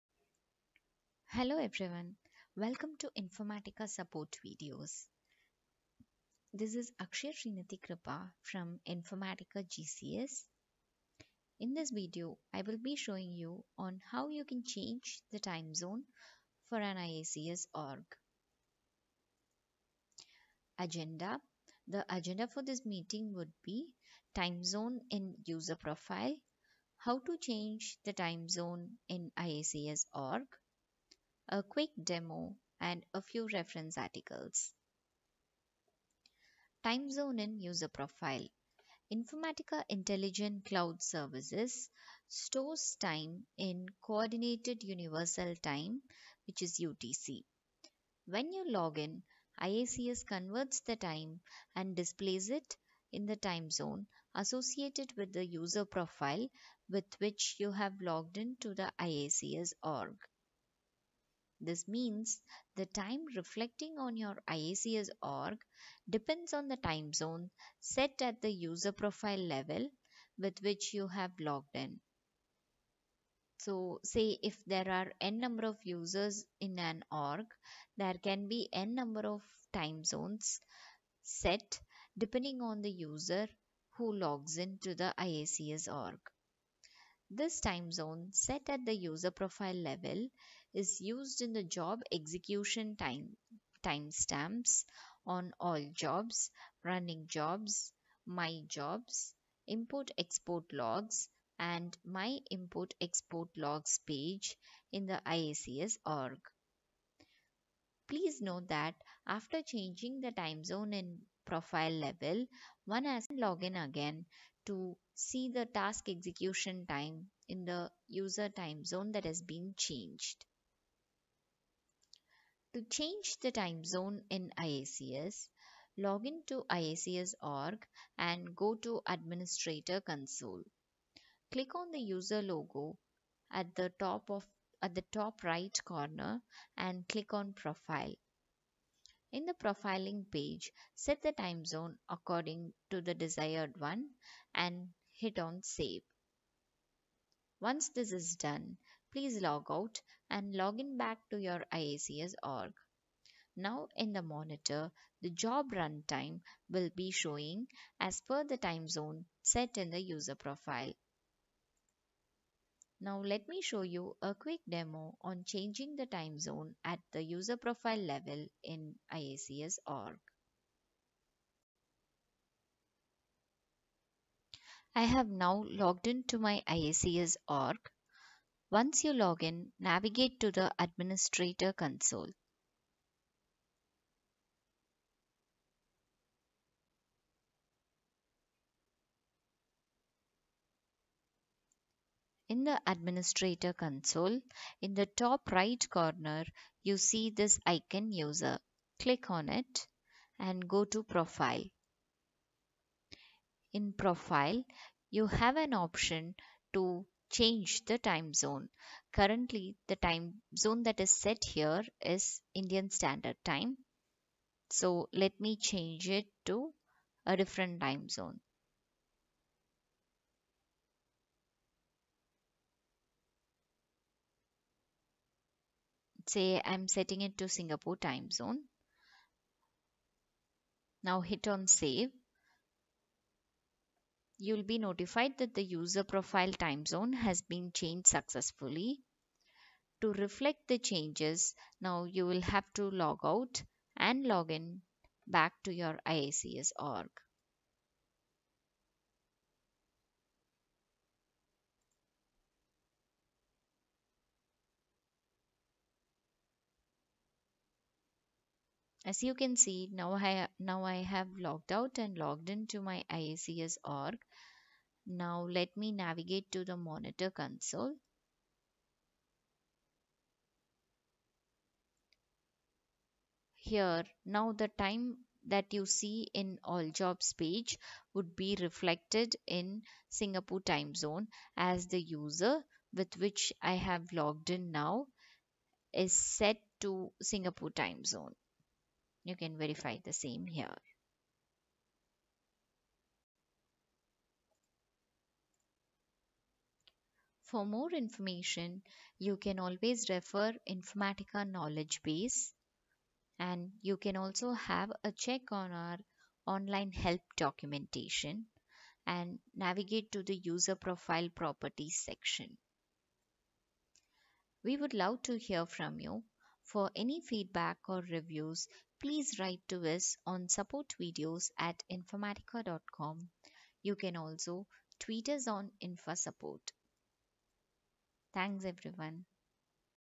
How to Change the Time Zone for IICS Org
This video explains how to change the time zone for IICS Org.

Expertise:  Beginner
User type: Administrator
Category:  Product usage
Project Phase: Configure